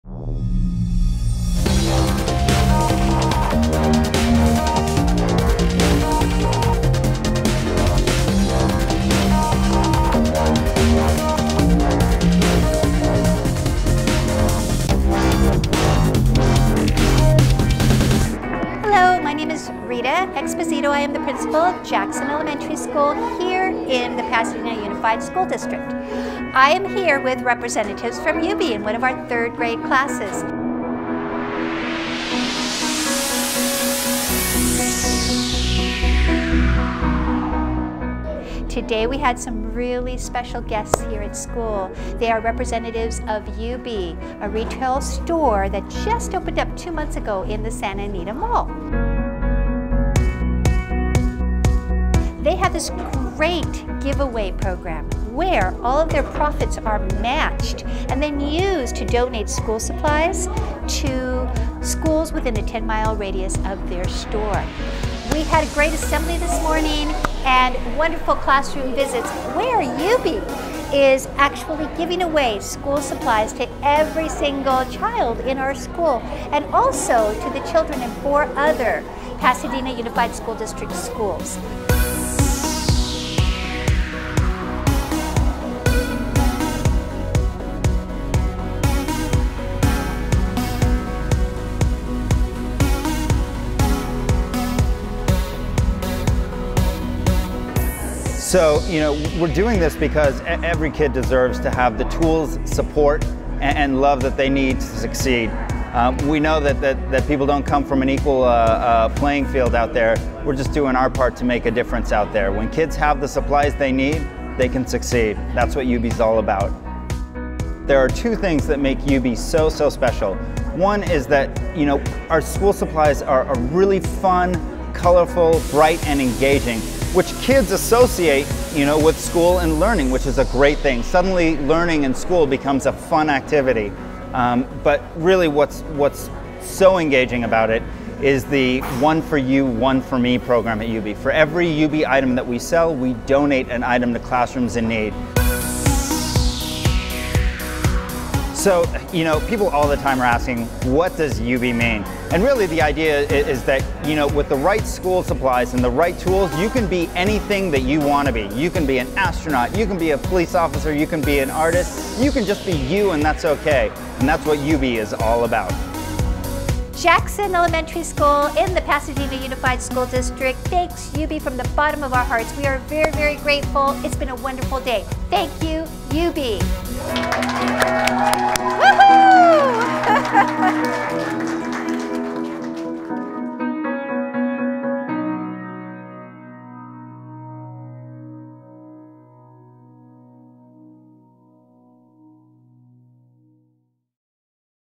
PUSD Pulse: Yoobi Donates to Jackson STEM Magnet School
Thank you to Yoobi's "One For You. One For Me" program for donating brand new school supplies to the students of Jackson STEM Magnet in Pasadena, Ca!